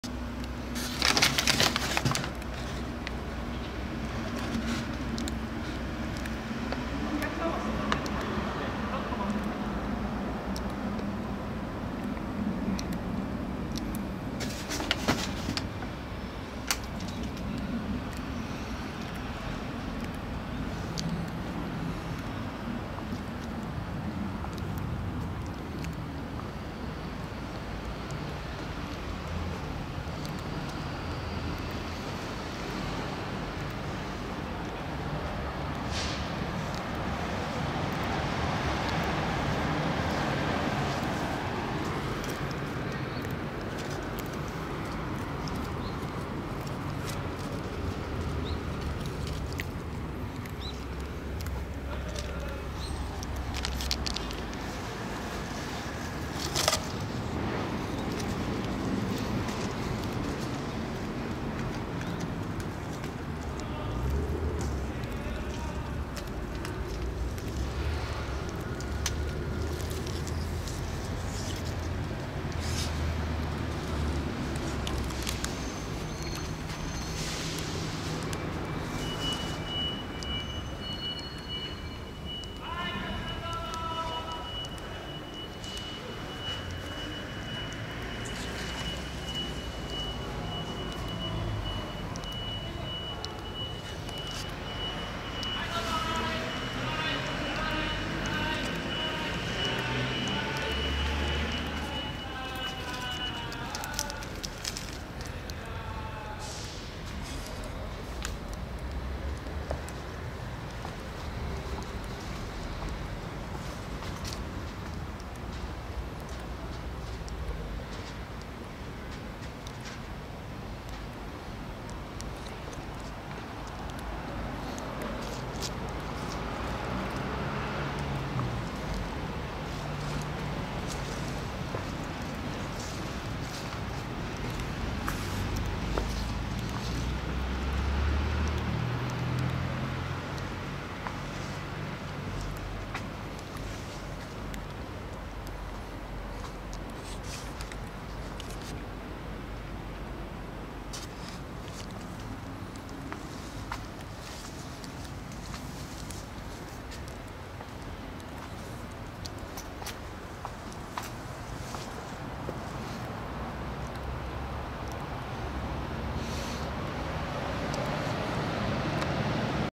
家の薔薇たちにスワルスキープラス UM（天敵：ガブリダニ）を設置 2018.12.1 昼 4k
スワルスキープラス UM